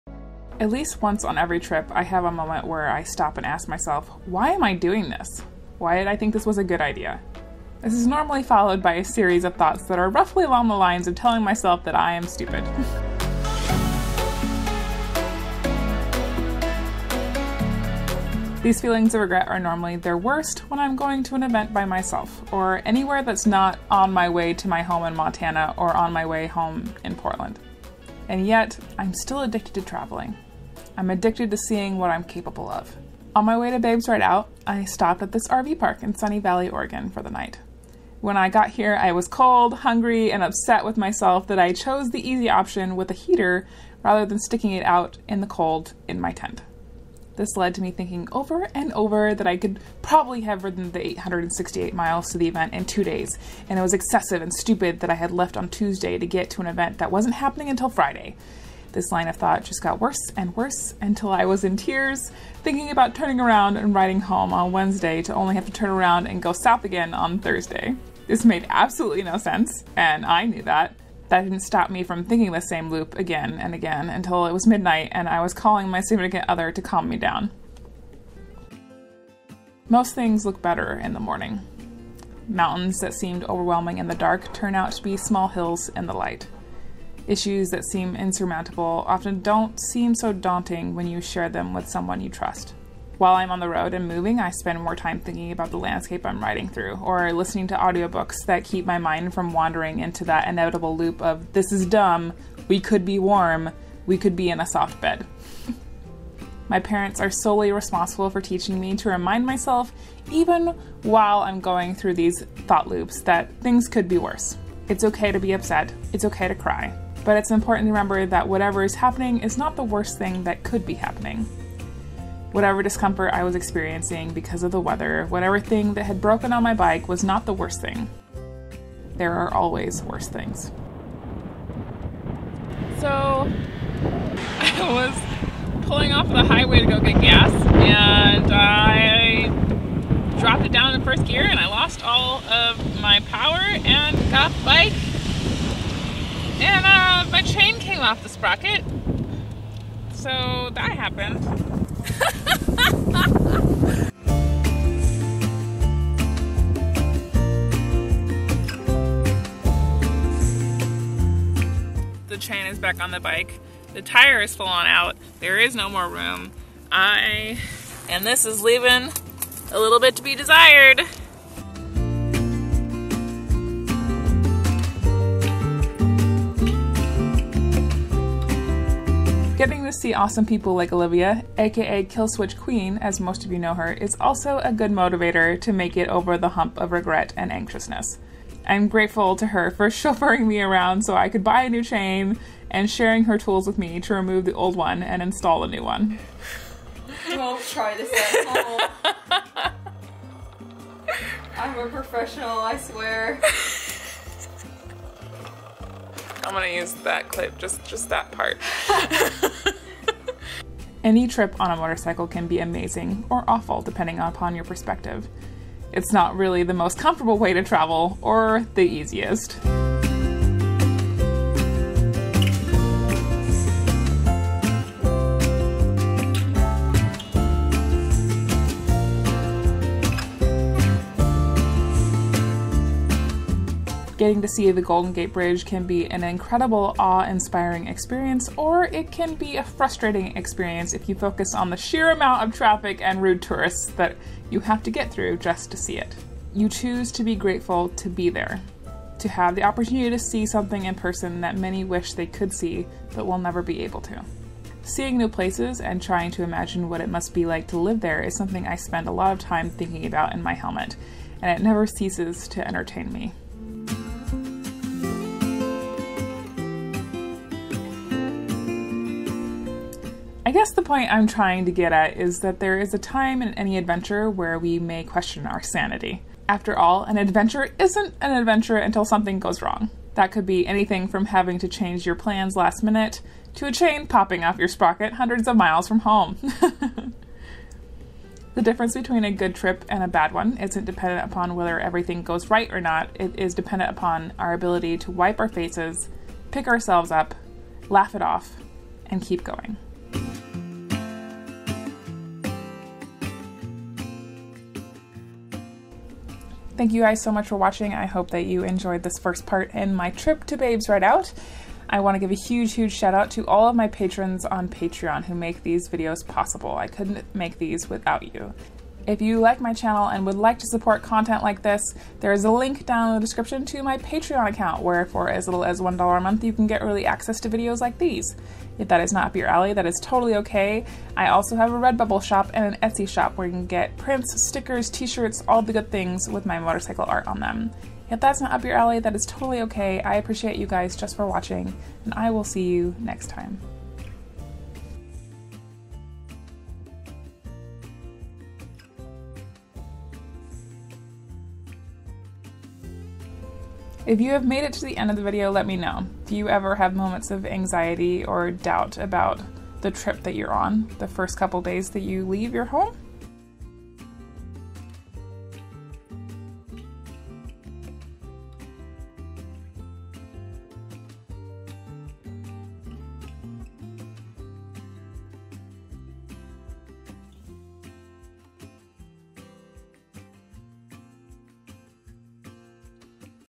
Overcoming Travel Anxiety | Thoughts from the Road
At least once on every trip I have a moment where I stop and ask myself “Why am I doing this, why did I think this was a good idea.” This is normally followed by a series of other anxious thoughts.

Tripped Travel Gear (Packing Cubes)
My New Wolfman Luggage
My new MSR Windburner Stove

ETSY SHOP (Original Drawings, Paintings and some Postcards)